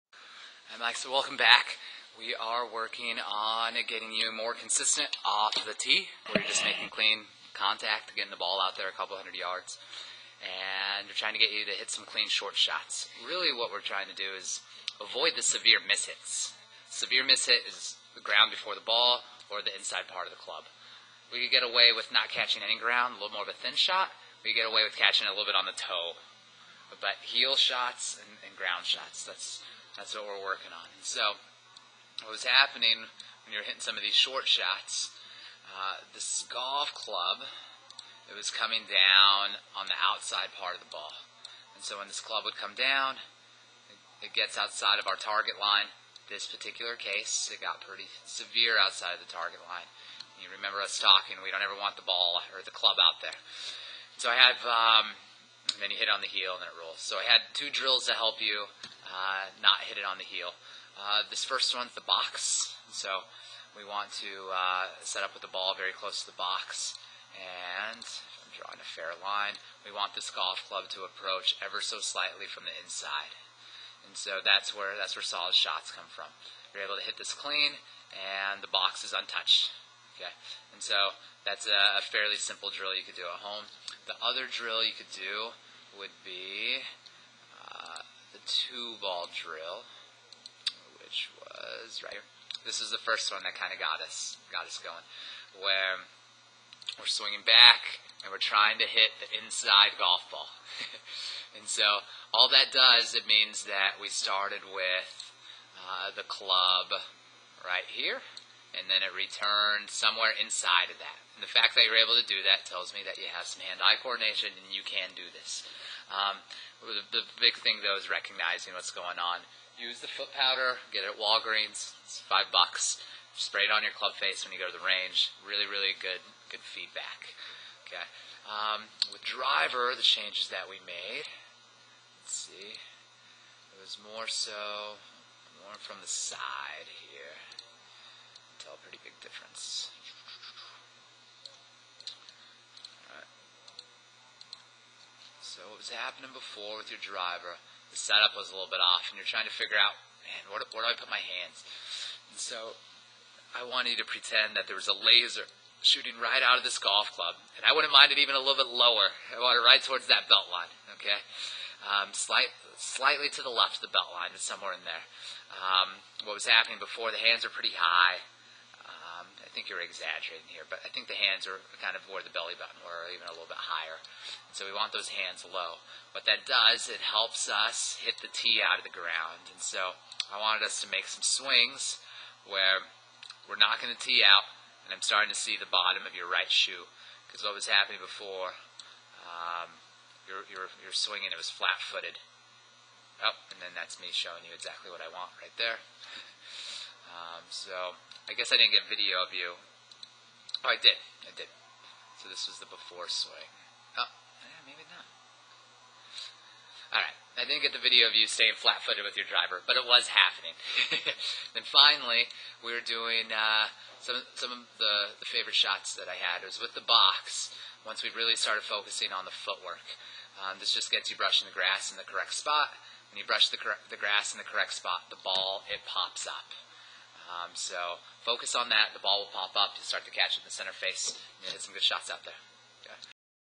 MY CHICAGO GOLF- Example Lesson- Driver and Short Game
(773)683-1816, analyzed by Marcus Yado, PGA: Michael is working on making clean contact with his driver and short game.